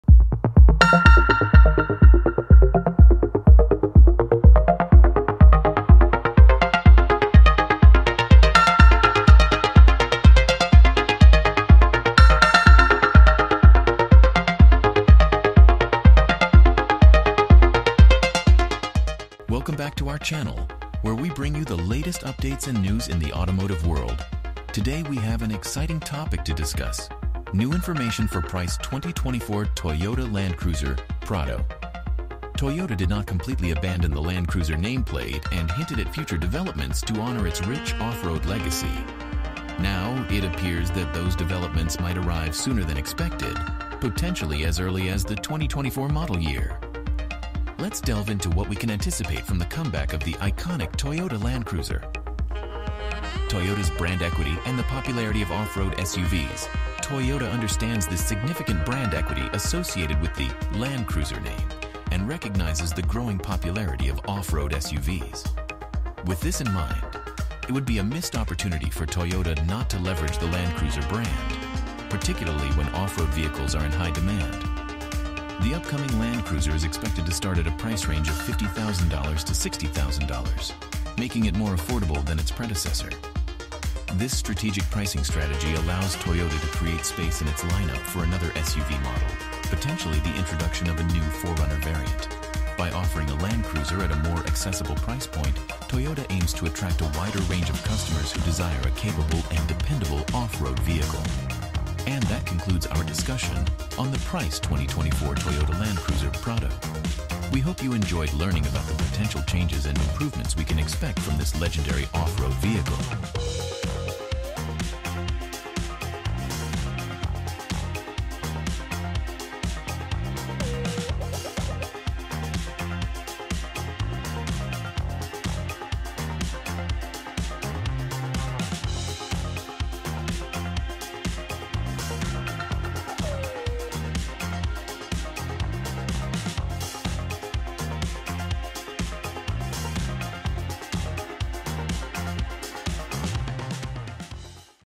2024 Toyota Land Cruiser Prado Price - NEW Information For Price Toyota Land Cruiser Prado
Today, we have an exciting topic to discuss New information for price 2024 toyota land cruiser prado.

Toyota did not completely abandon the Land Cruiser nameplate and hinted at future developments to honor its rich off-road legacy. Now, it appears that those developments might arrive sooner than expected, potentially as early as the 2024 model year. Let's delve into what we can anticipate from the comeback of the iconic Toyota Land Cruiser.